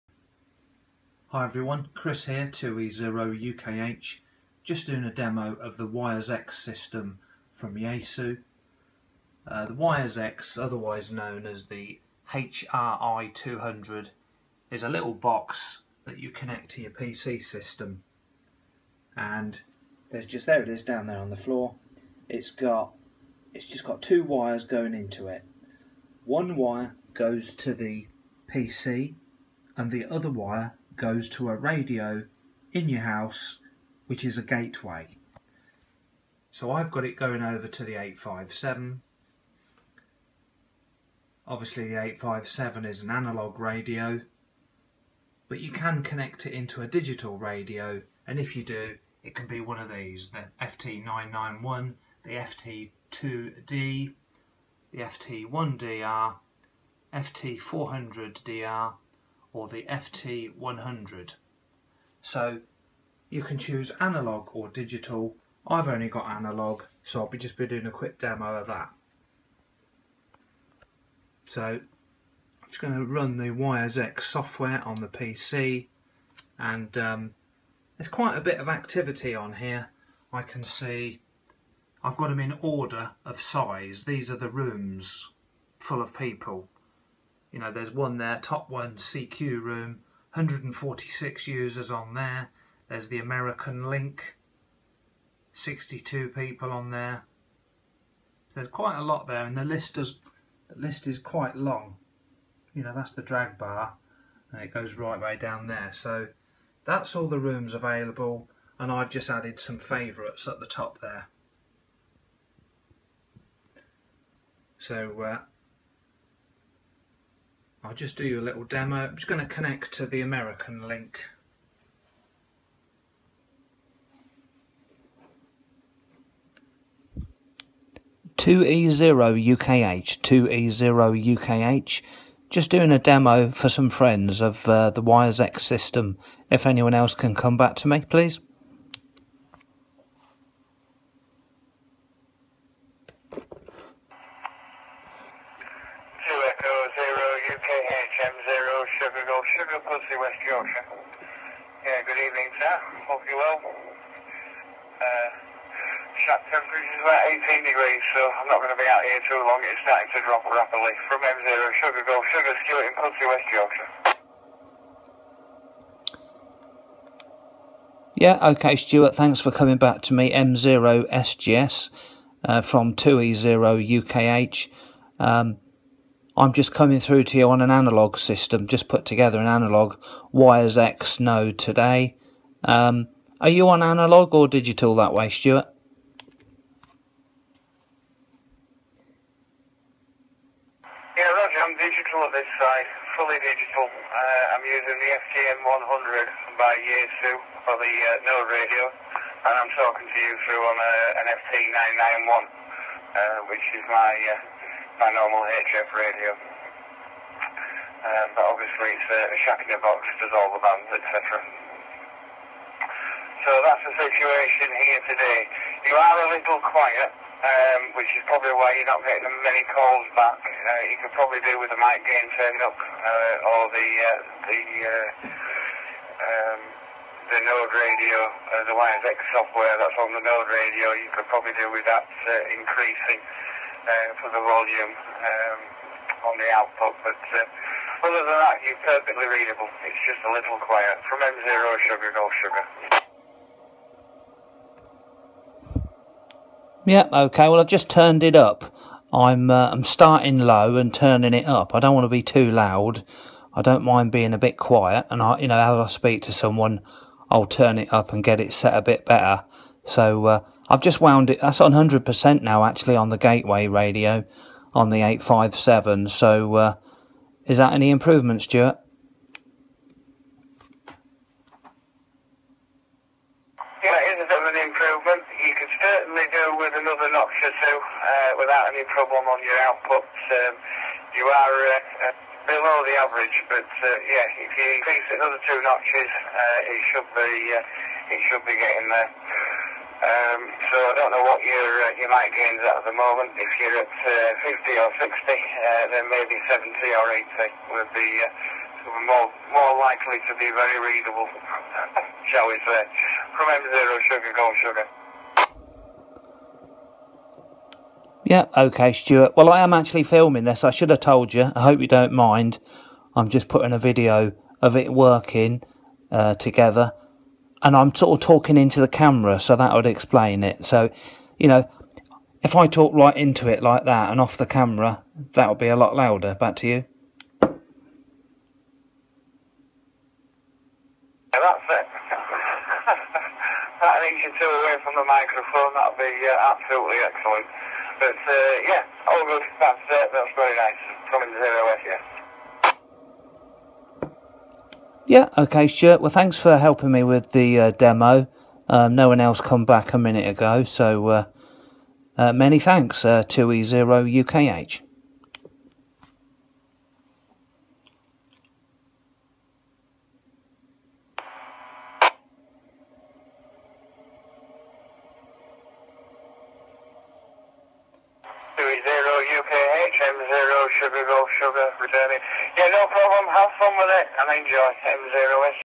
Yaesu wires x analogue demo
Here is a quick demo of the wires X system. The box is connected to a PC and also an analog radio. This radio becomes the Gateway and you access it via a FM radio in your house.